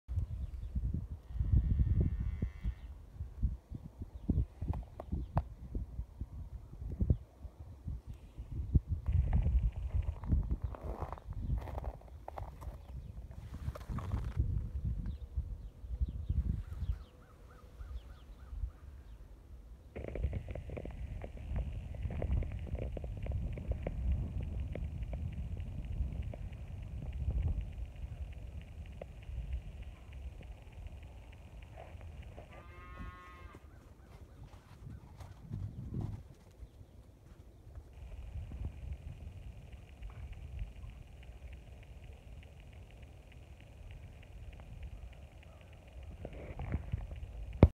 Video 3 Using Nose Flap Weaning
Day 7 after placing the flaps, They come out today! And will be fence line separated from the cows so we can better care for them.